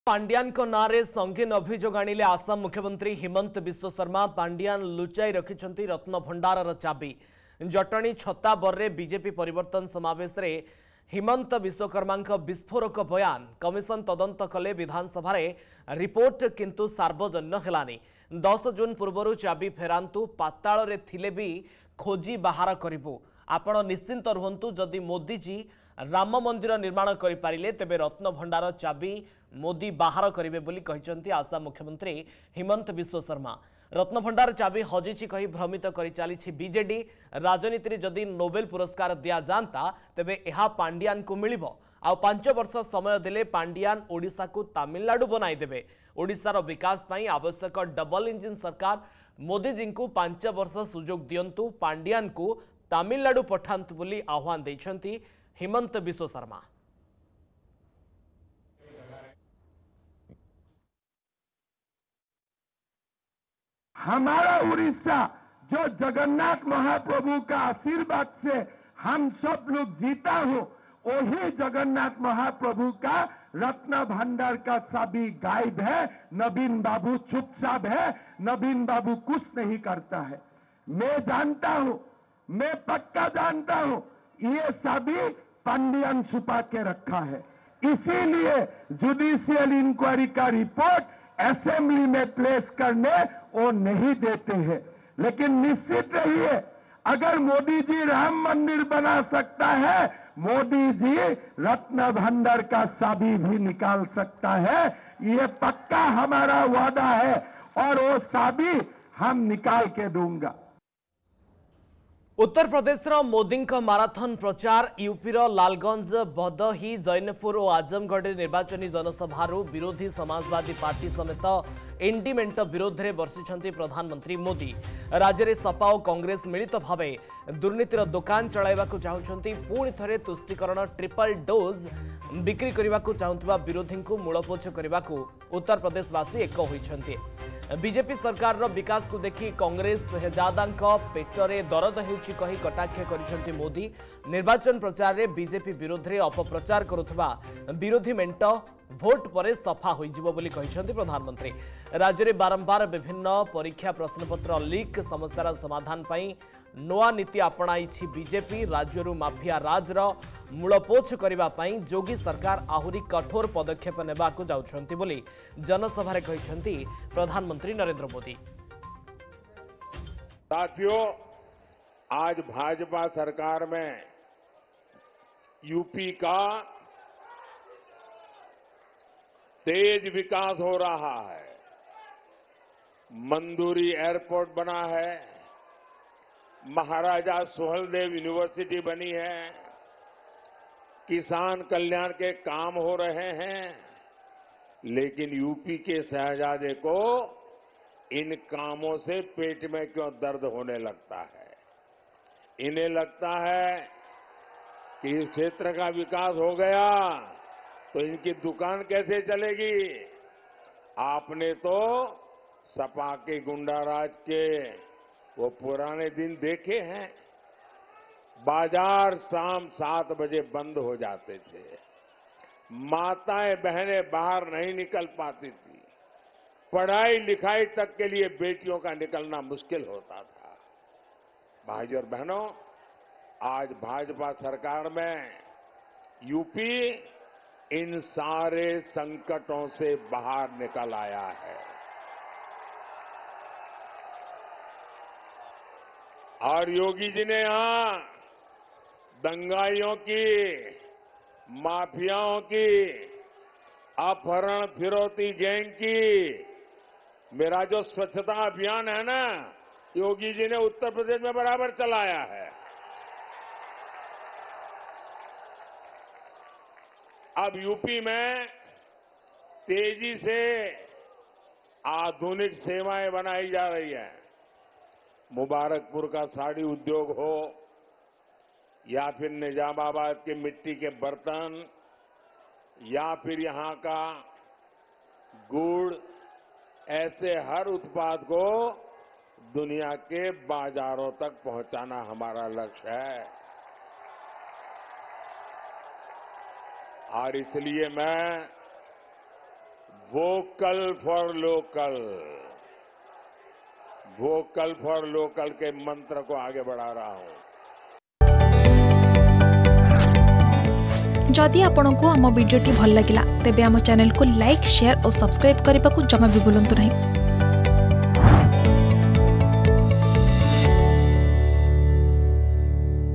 ପାଣ୍ଡିଆନ୍ ଲୁଚାଇ ରଖିଛନ୍ତି ରତ୍ନଭଣ୍ଡାର ଚାବି || Himanta Biswa Sarma || V K Pandian
ପାଣ୍ଡିଆନଙ୍କ ନାଁରେ ସଂଗୀନ ଅଭିଯୋଗ ଆଣିଲେ ଆସାମ ମୁଖ୍ୟମନ୍ତ୍ରୀ ହିମନ୍ତ ବିଶ୍ବଶର୍ମା । ପାଣ୍ଡିଆନ୍ ଲୁଚାଇ ରଖିଛନ୍ତି ରତ୍ନଭଣ୍ଡାର ଚାବି ।